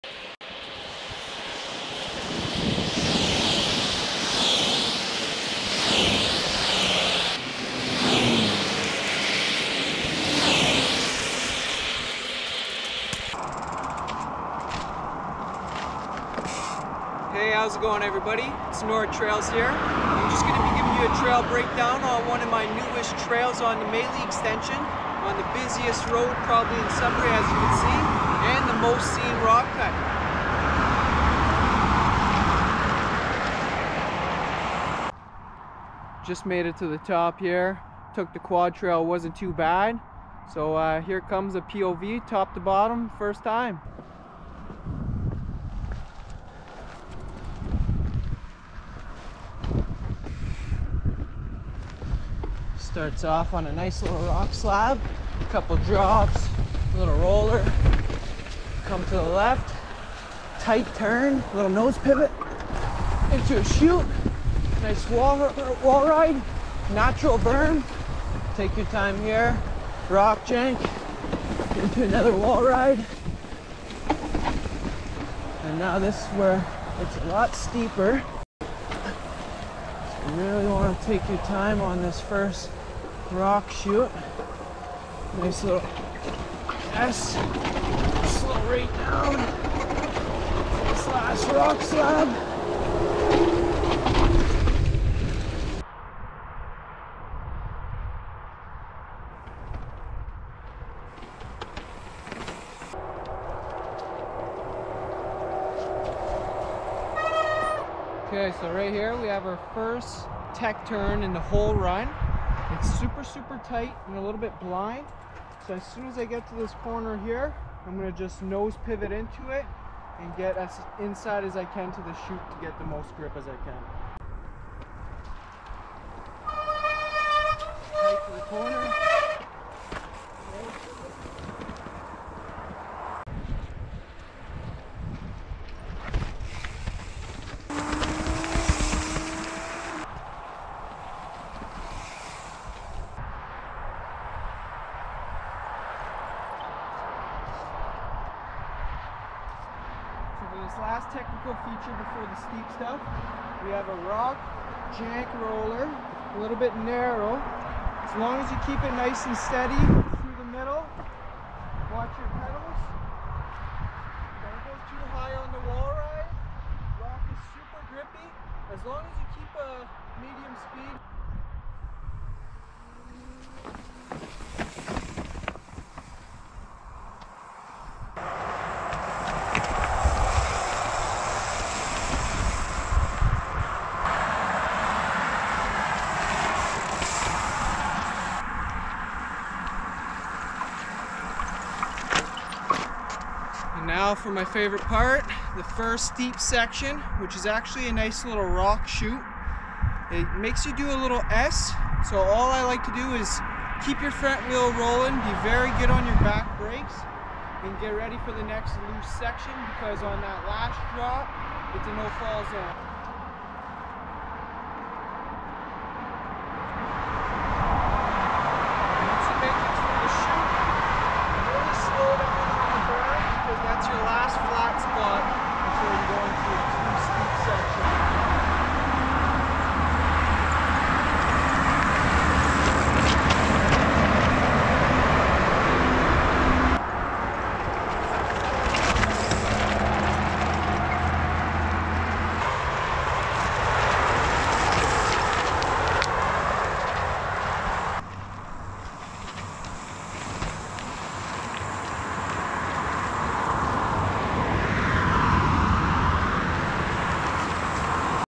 New Trail Breakdown / Sudbury Maley POV / Steep Tech Loose
New trail that was just finished on the Maley extension in Sudbury Ontario on one of the biggest rock cuts. Full breakdown and POV on my devinci spartan from Sessions